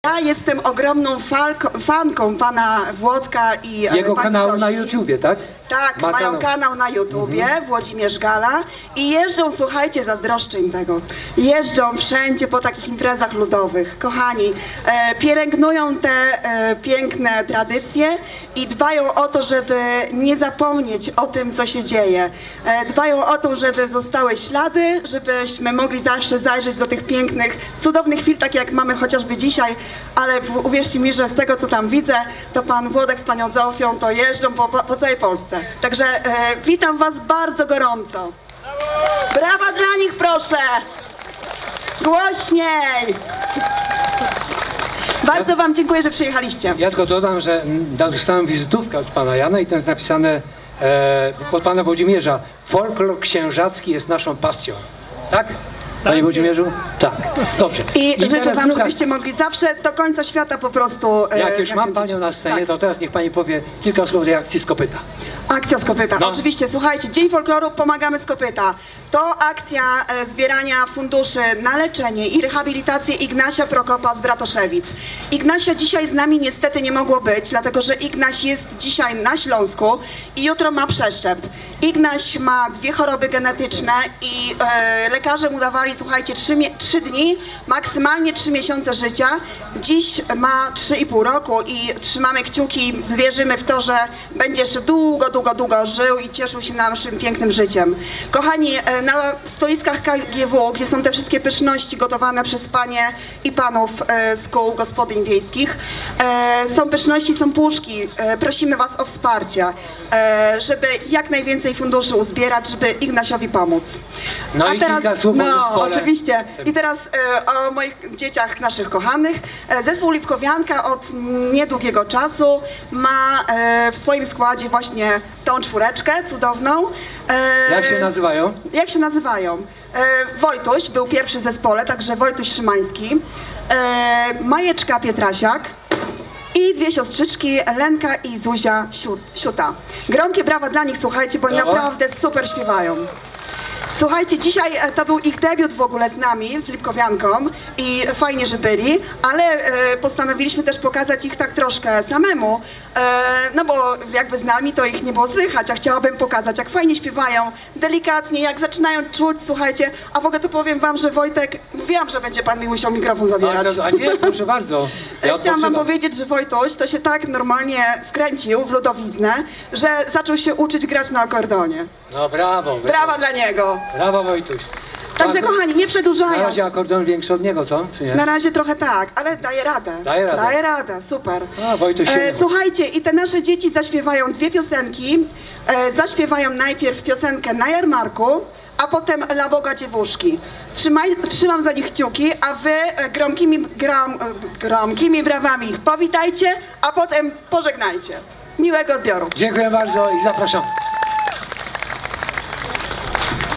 DZIĘKUJEMY PANI MARZENKO.
Dziękujemy Pani Marzenko za miłe słowa pod naszym adresem. Tak. , folklor, nasza wieś, nasze tradycje, nasza kultura jest naszą pasją. Sami jesteśmy częścią tej kultury, jesteśmy "Księżakami"z krwi i kości ". Nasza pasja przeradza się w materialne utrwalanie tego wszystkiego , co z tą kulturą jest związane, a więć póki sił starczy, będziemy filmować i fotografować dla potomnych.